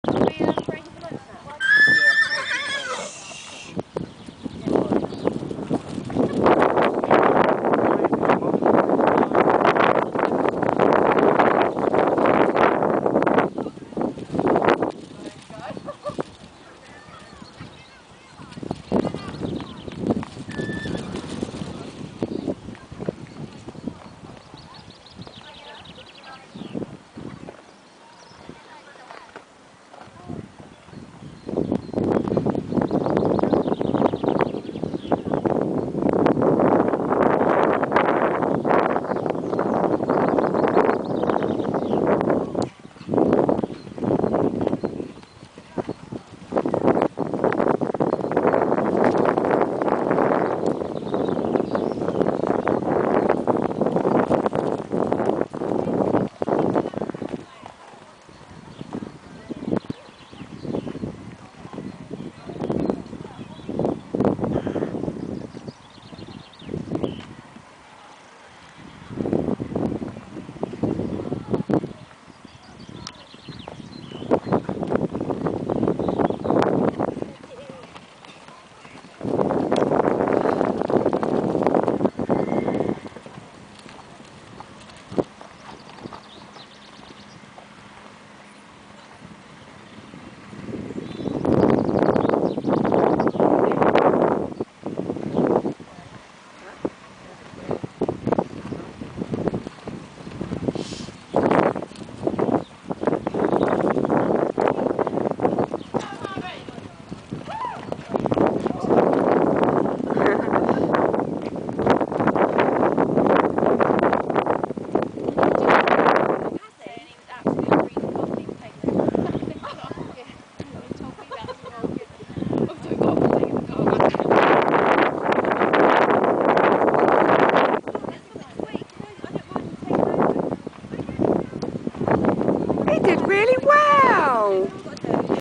Harvey show 16/05/2010
I took Harvey to a show and this is it haaa.
Not edited it, just uploaded as it is!
He was absoloutly incredible. Faultless! We haven't competed since July last year, we haven't jumped a course or jumped fillers since July last year! Andd he's been injured for 3 months of this year, so we're back on top form now I think! He went totally clear around it. Im so proud of him! He loved it and just woww :)
Me im not so amazing as I've only had him back jumping for 2 weeks since the accident! But i had a lovely day and im on a high now from it!

Absoloute no crit as I could not be happier with the boy :)